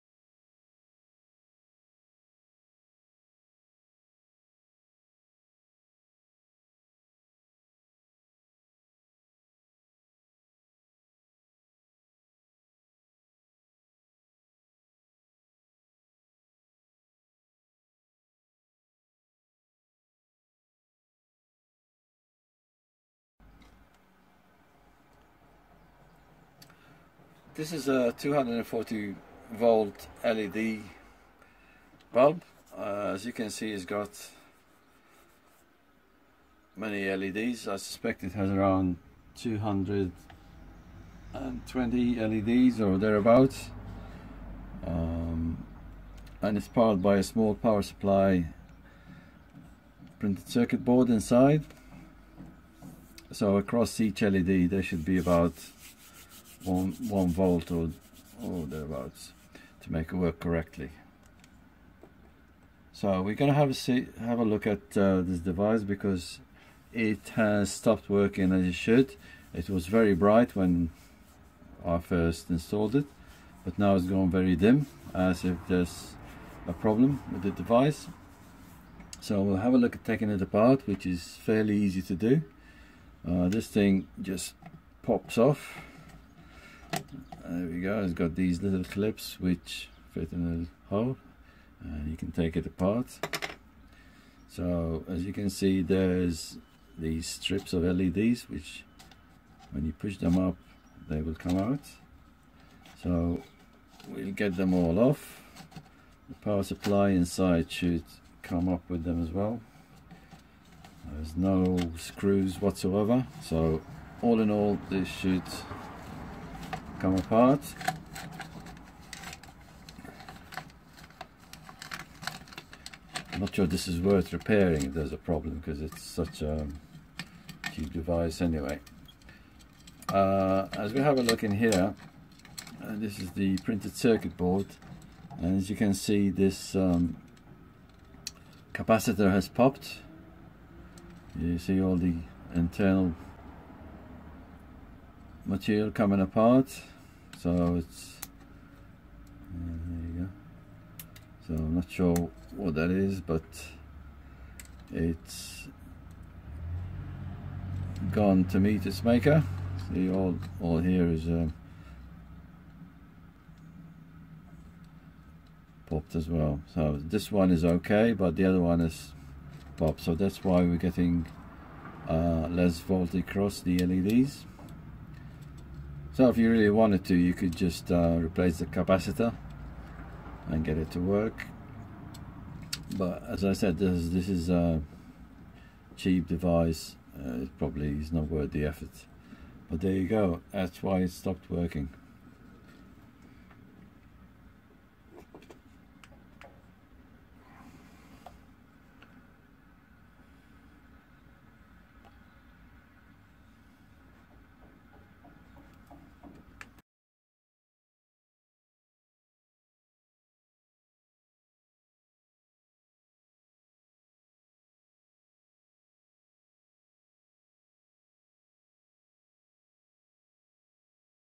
Why LED Bulb is very dim
LED bulbs are very command in the household nowadays. I have notices on several occasions that bulbs stop being as bright as they should be. Why is this. opening up the bulb will expose the reason for this.